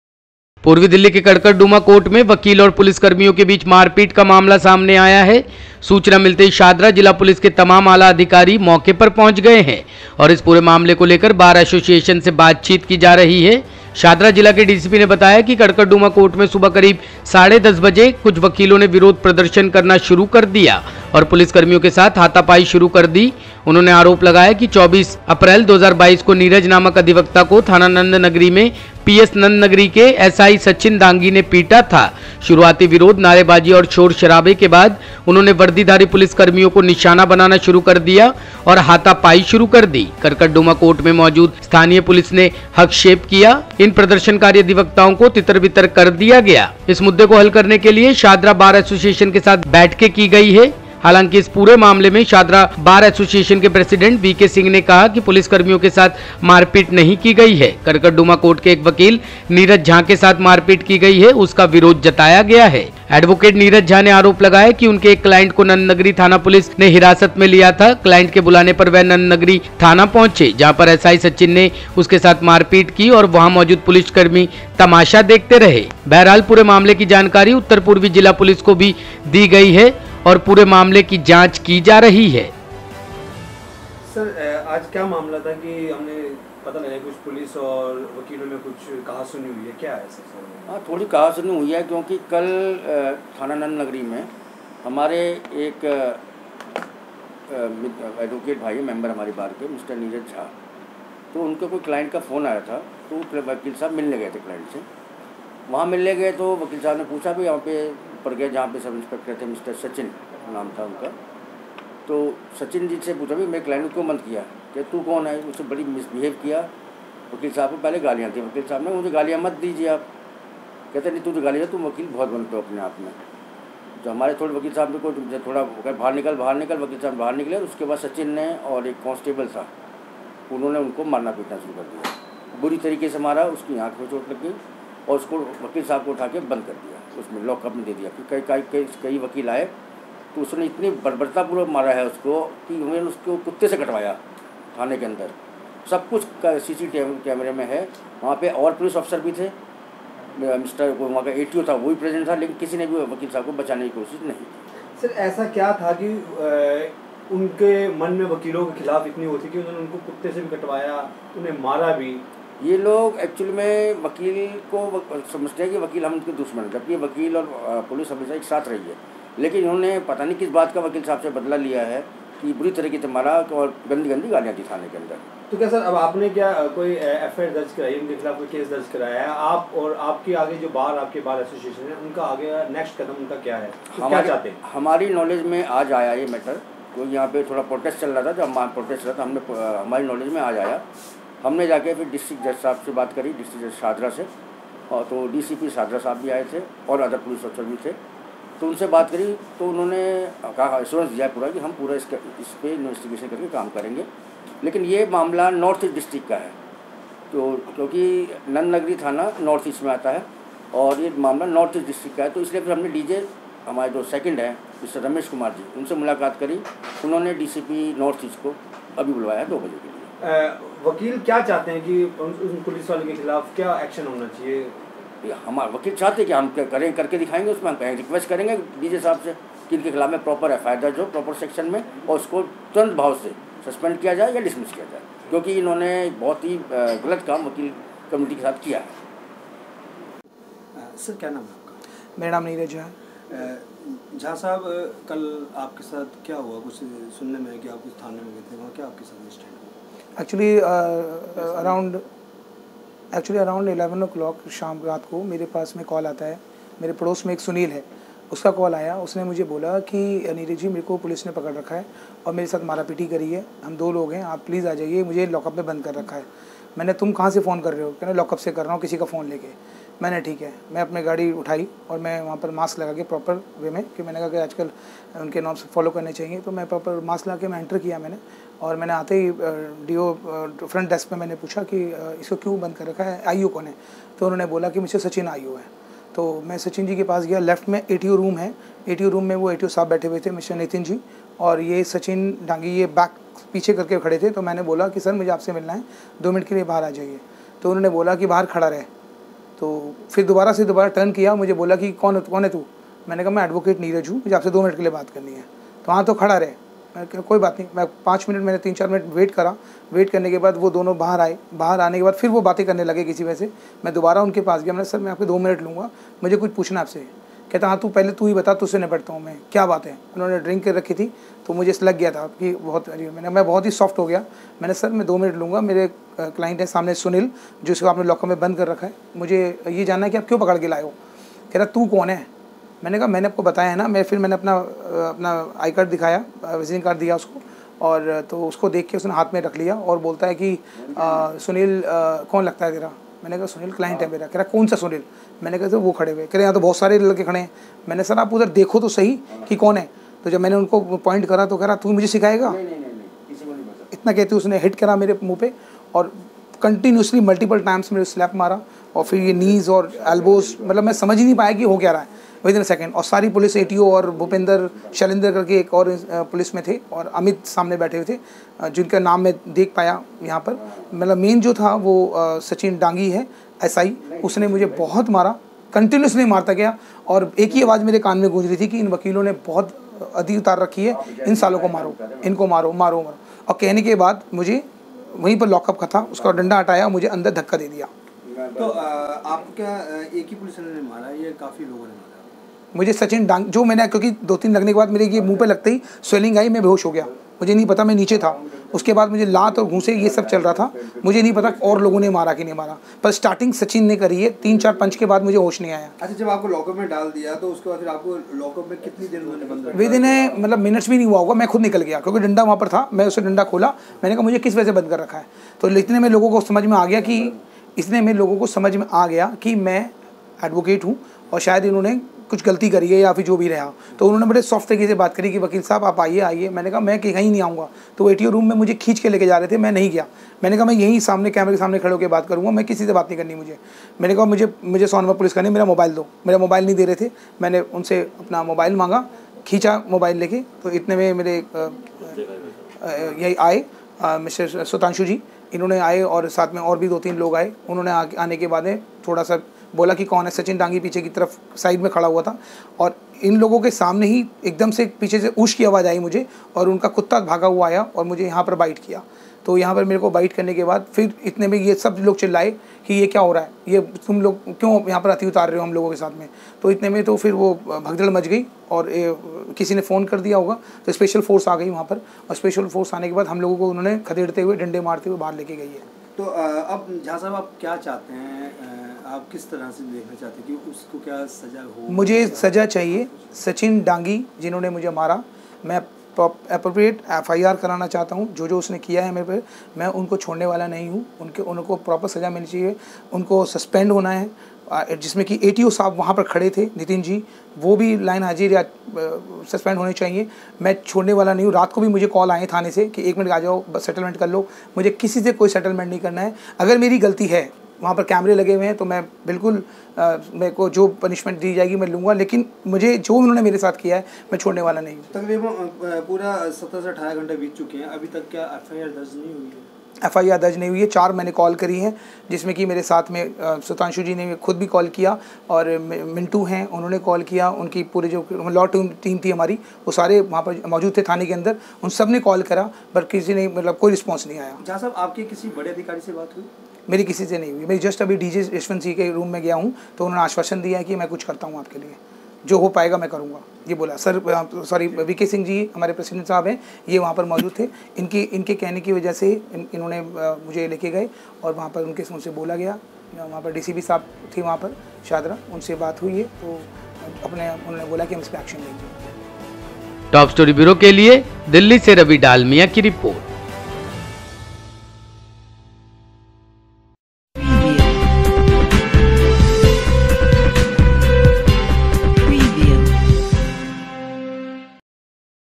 कड़कड़डूमा कोर्ट में वकील और पुलिसकर्मियों के बीच मारपीट,मामले के निपटारे में जुटे दोनों पक्ष
पूर्वी दिल्ली के कड़कड़डूमा कोर्ट में वकील और पुलिसकर्मियों के बीच मारपीट का मामला सामने आया है सूचना मिलते ही शाहदरा जिला पुलिस के तमाम आला अधिकारी मौके पर पहुंच गए हैं और इस पूरे मामले को लेकर बार एसोसिएशन से बातचीत की जा रही है.